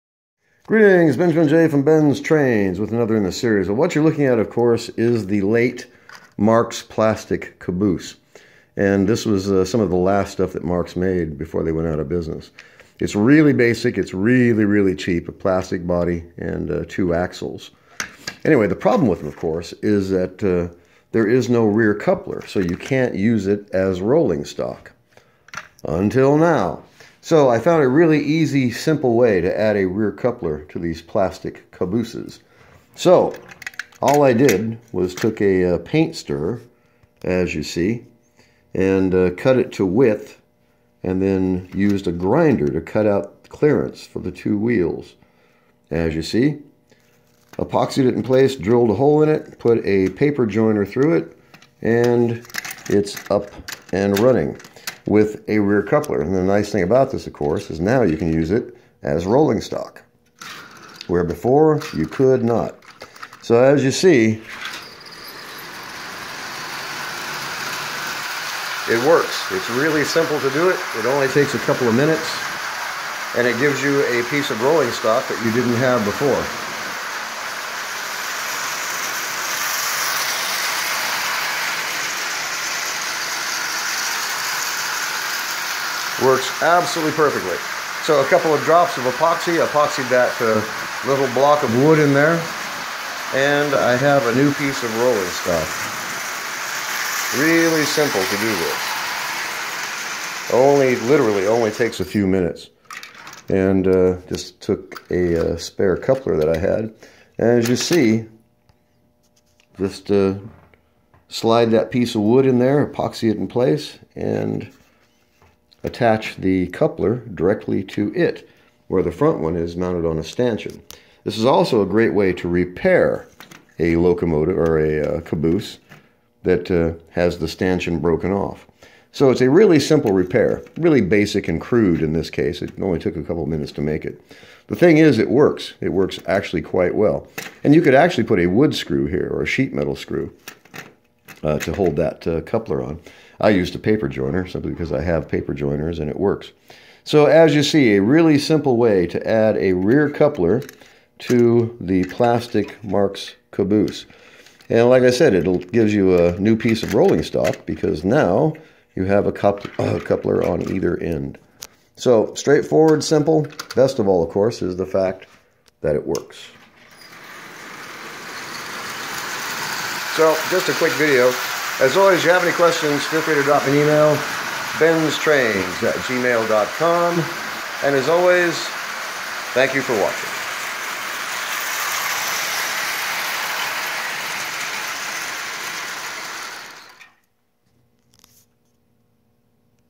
MARX LATE PLASTIC CABOOSE / ADDING A REAR COUPLER / WORKS
crude but effective, also an excellent easy way to repair a plastic caboose with a broken or missing coupler.